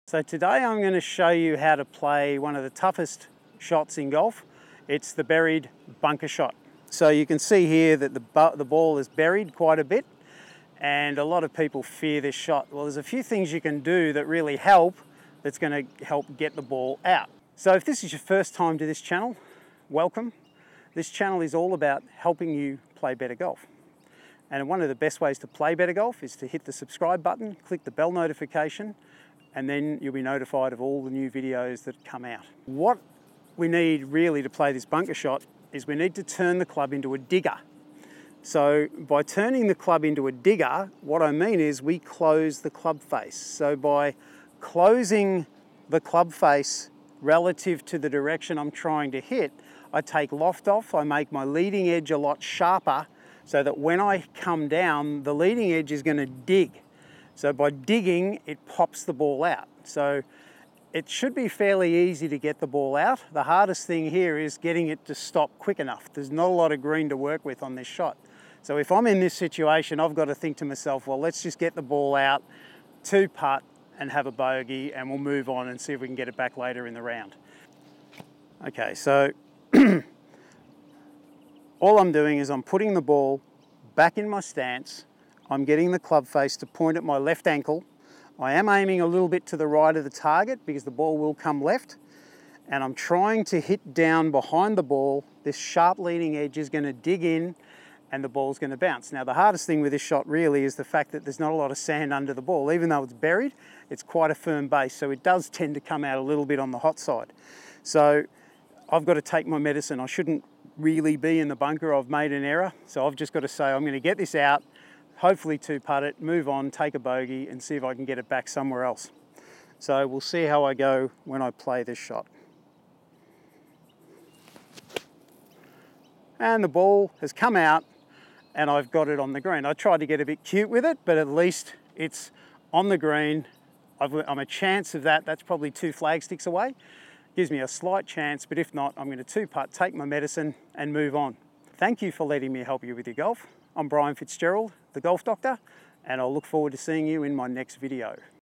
Buried Bunker Shot Technique
Buried bunker shot technique. In this weeks golf tips video I show you how to play a buried bunker shot. Bunker shots are scary at the best of times but this need not be the case.But with a simple set up adjustment you will get the ball out of the buried bunker shots every time